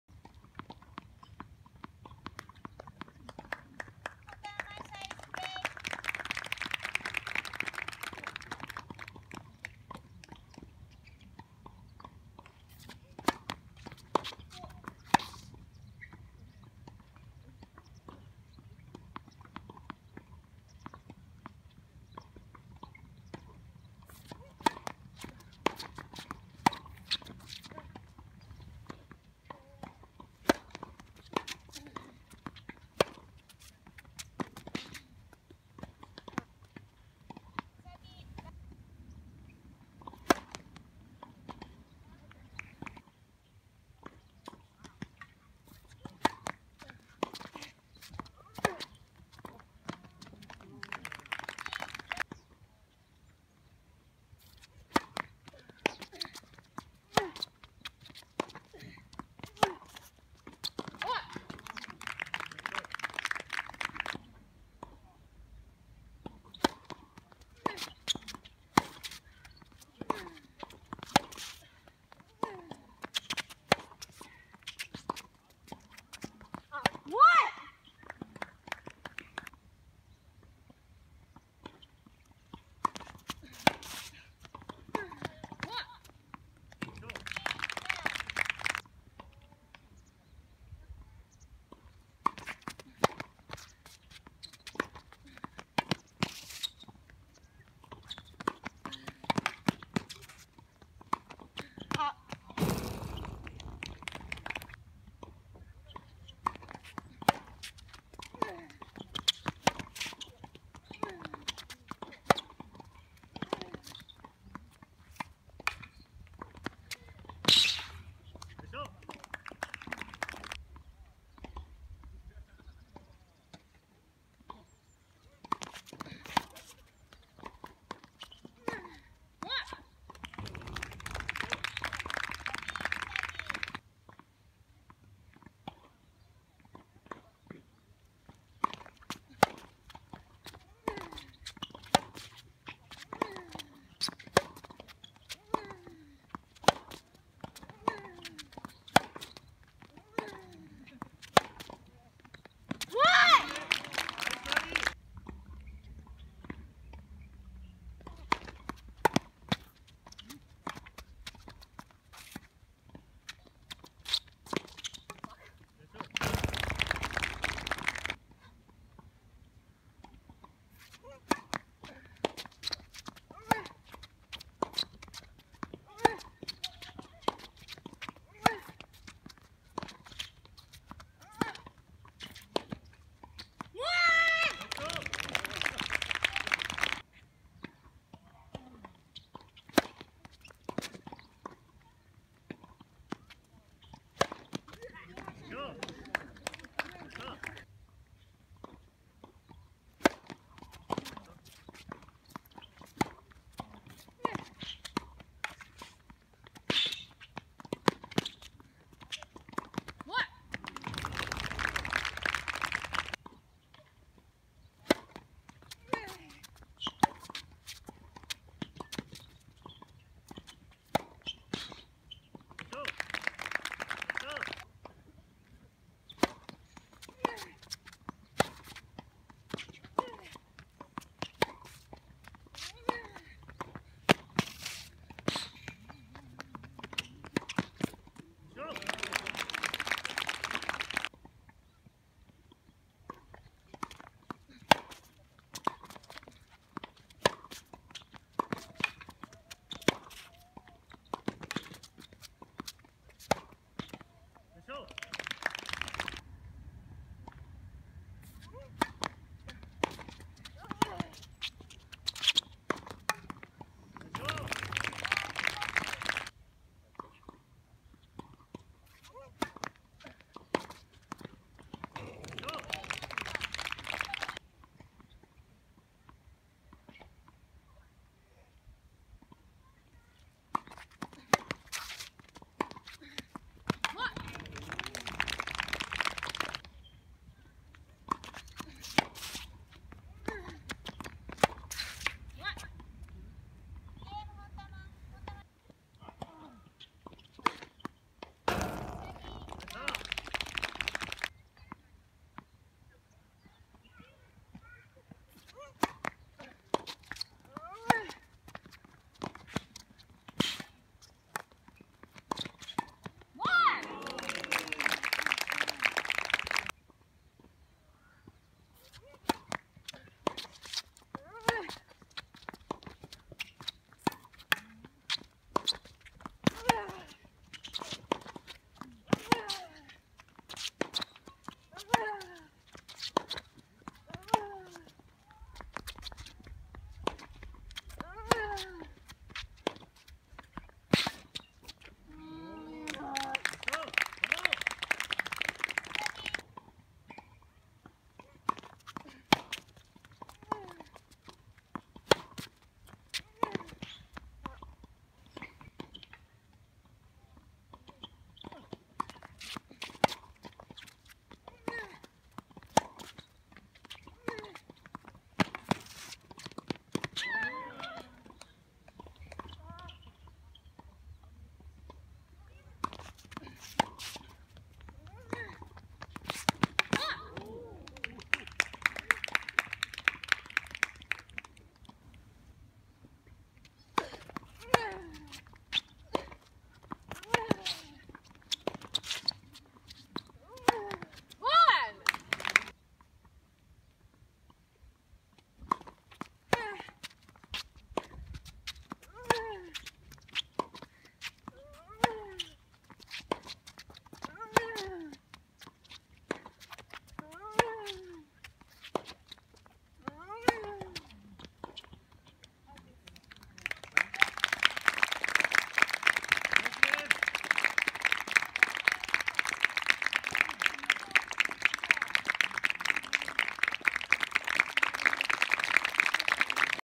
好プレー集！【高崎オープン2020】秋田史帆 vs 本玉真唯 女子シングルス決勝戦
高崎オープン2020
女子シングルス決勝戦
秋田史帆 64 64 本玉真唯